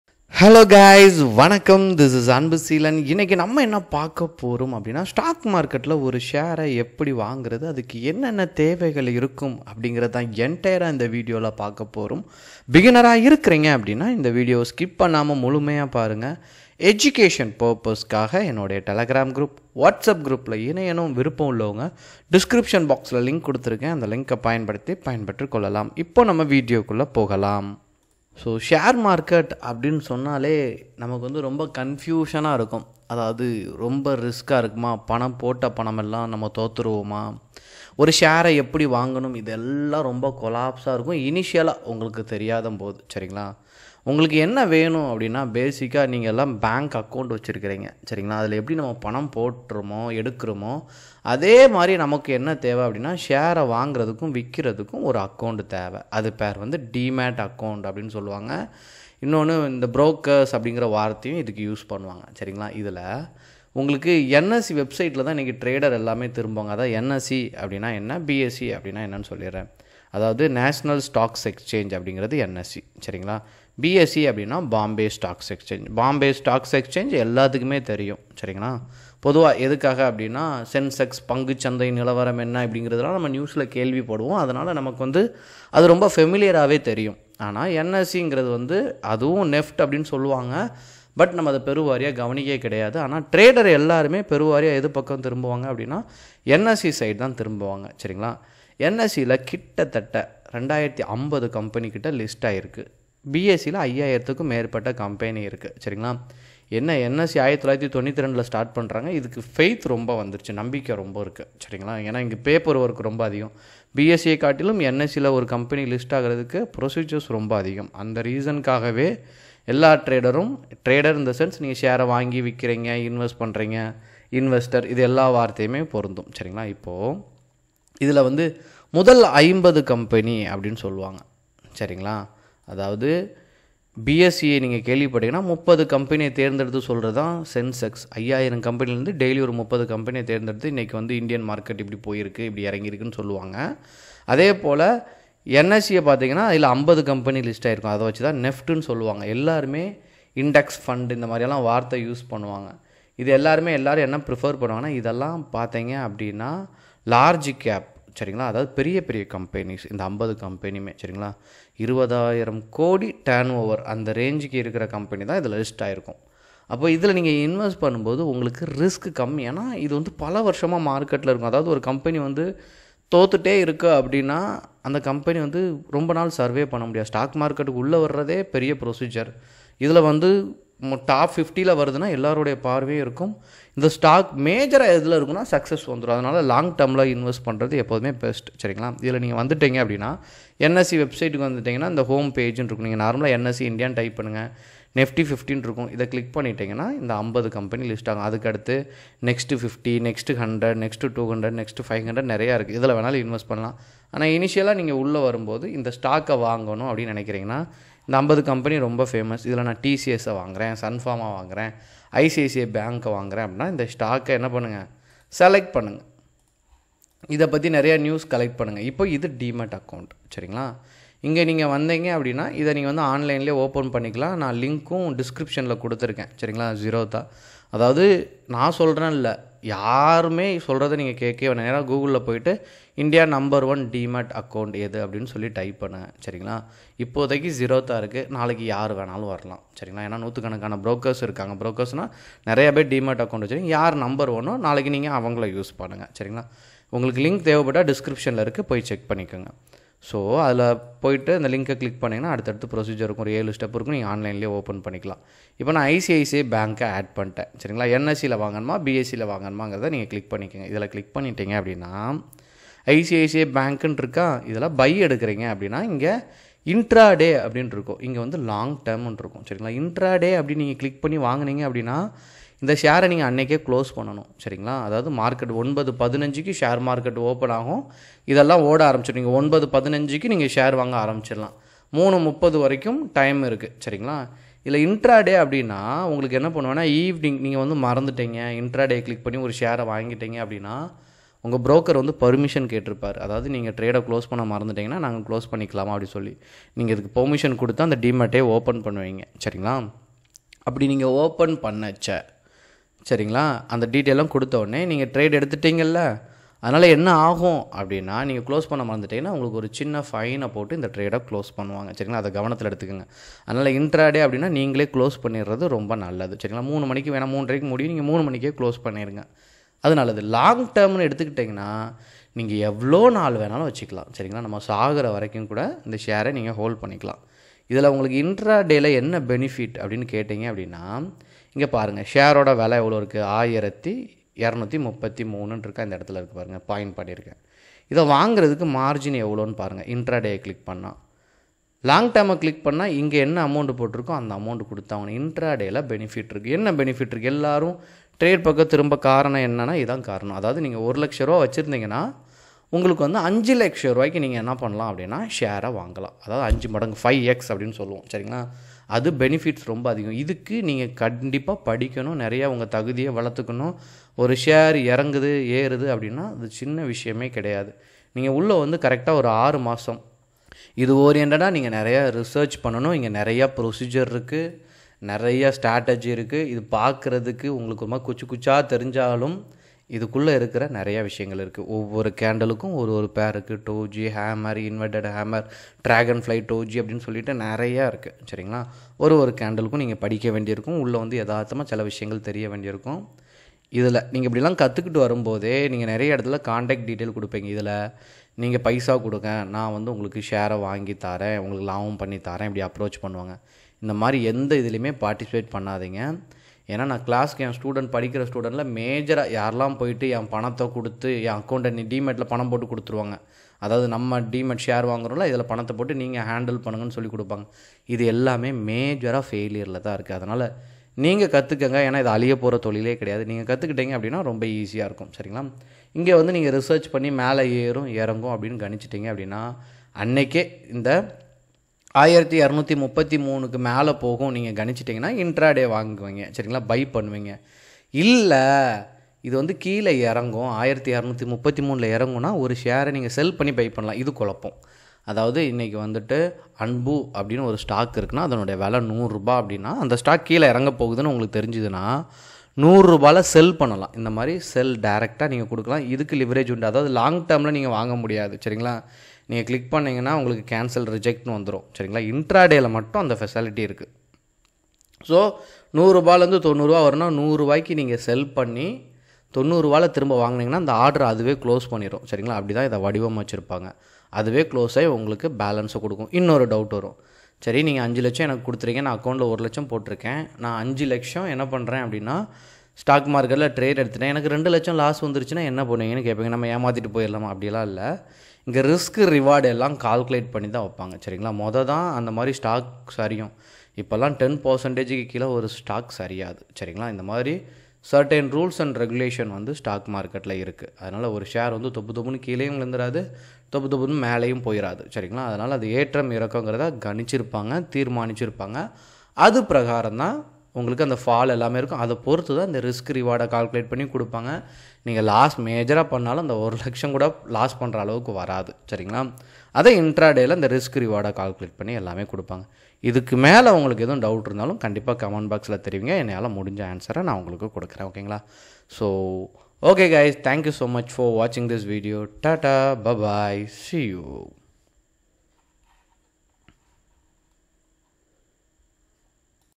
How to buy share? | Stock market basics for beginners in Tamil | what is share market?
JPA- Free class videos :

Class I

Class II

Class III

Class IV

MUST WATCH ALL VIDEOS (ONE LINK)

Class V